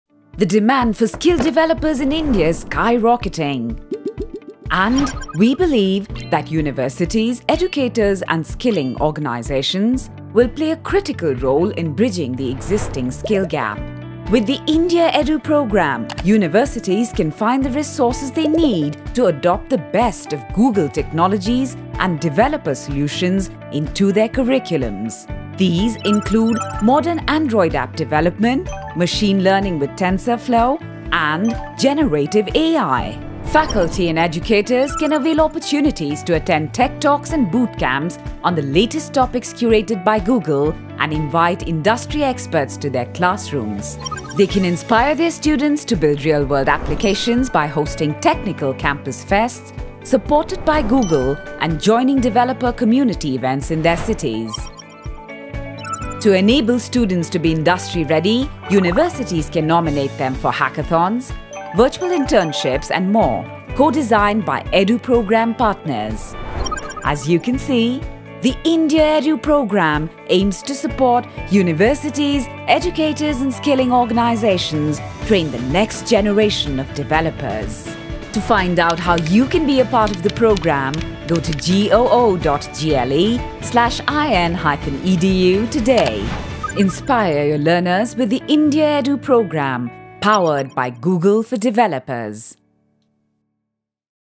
Welcome to India Edu Program by Google for Developers
The India Edu program by Google for Developers supports universities, educational organisations & educators with learning resources, faculty development and industry academia connect to train the next generation of developers & bring the latest Google technologies and developer solutions  like Android, Machine Learning with Tensor Flow, Generative AI and more into their classrooms.

Learn more about the India Edu Program by Google for Developers and be a part